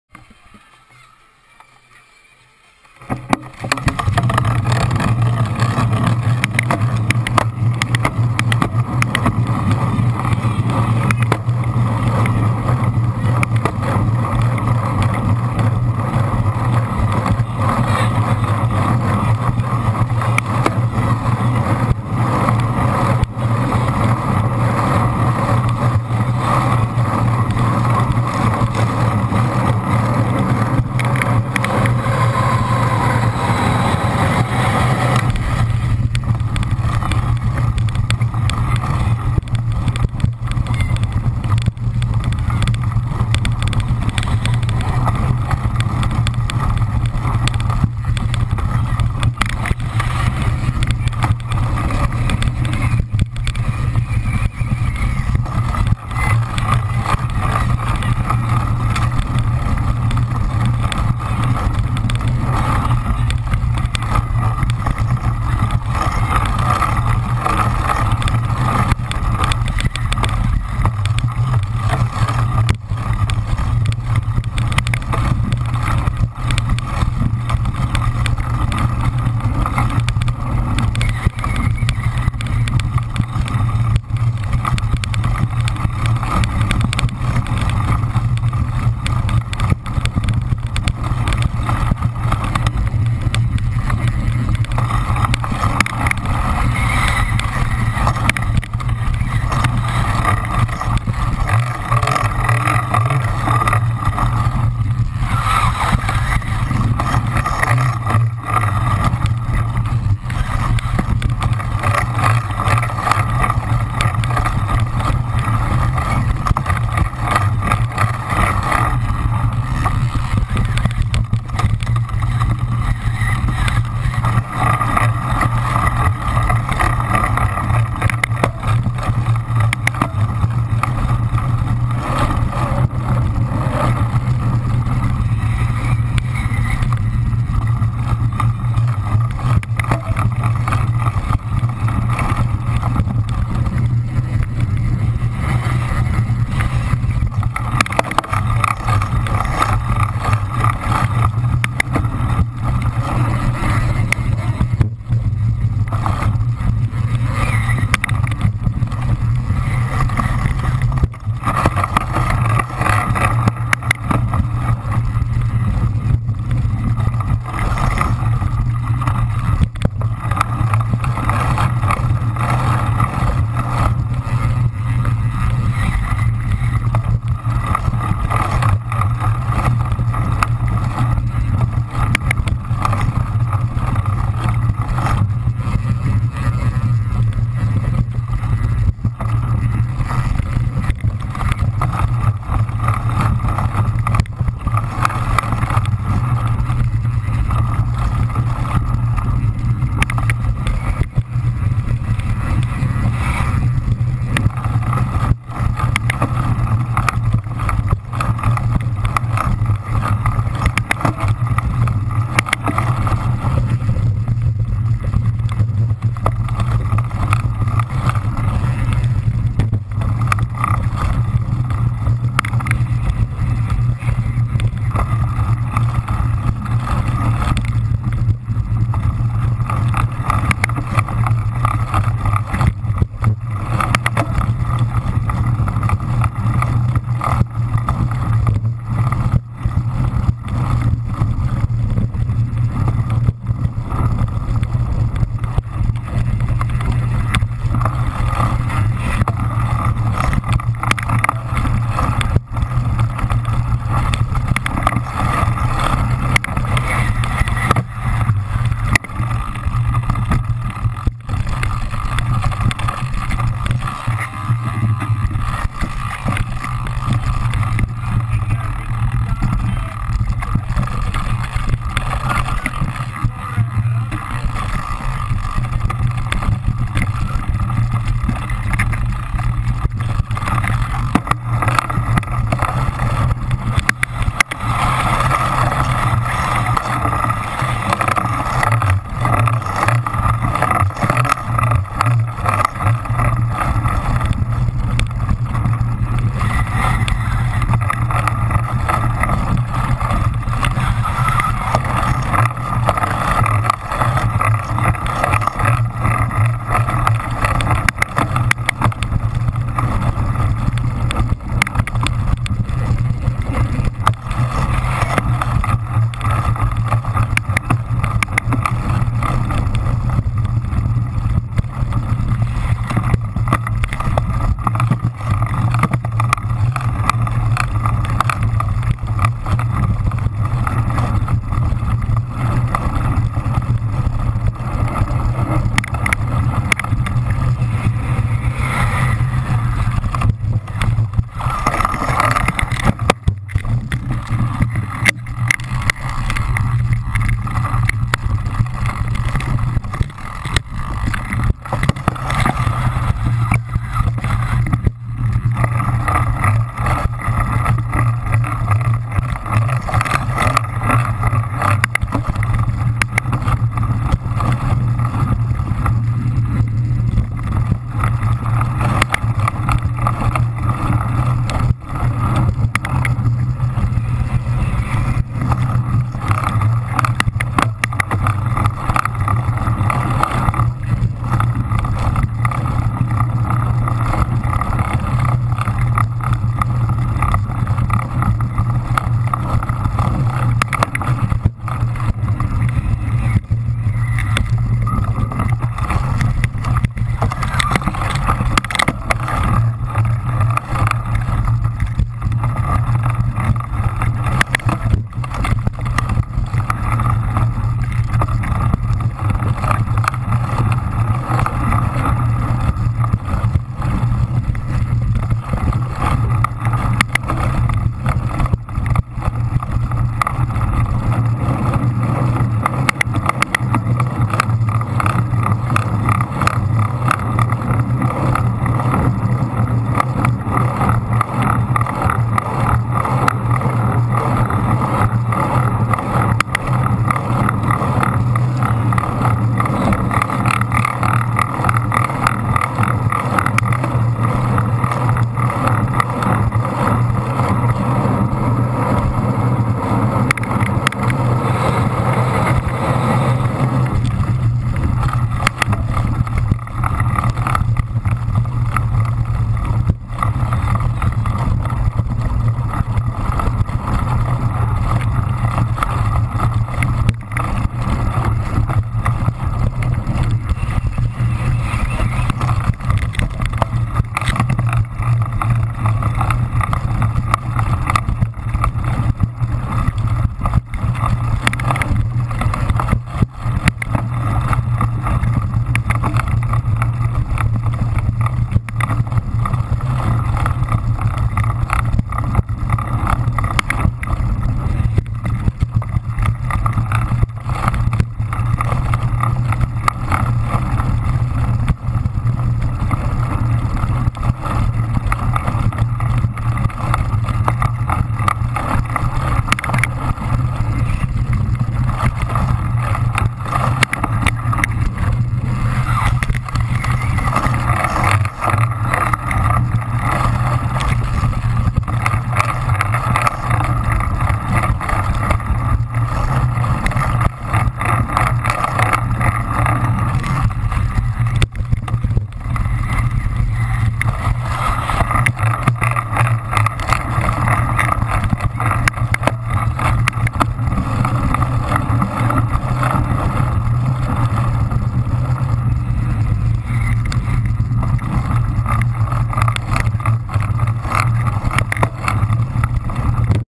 Sluitingsprijs Oostmalle 2012
The Oostmalle course is one of the most interesting and fun courses in the hole UCI calendar. It's a fast, twisty but also open course with some pretty sandy parts. It's absolutely not suiting me but I enjoyed riding this course every year.